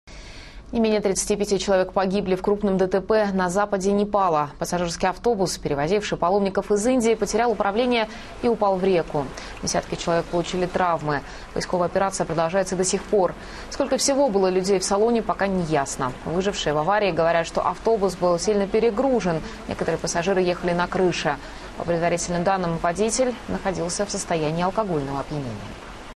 крупное ДТП в Непале, 35 погибших
В крупном ДТП на западе Непала погибли не менее 35 человек. Пассажирский автобус, перевозивший паломников из Индии, потерял управление и упал в реку. Десятки человек получили травмы. Поисковая операция продолжается до сих пор. Сколько всего было людей в салоне -- пока не ясно. Выжившие в аварии говорят, что автобус был сильно перегружен, некоторые пассажиры ехали на крыше.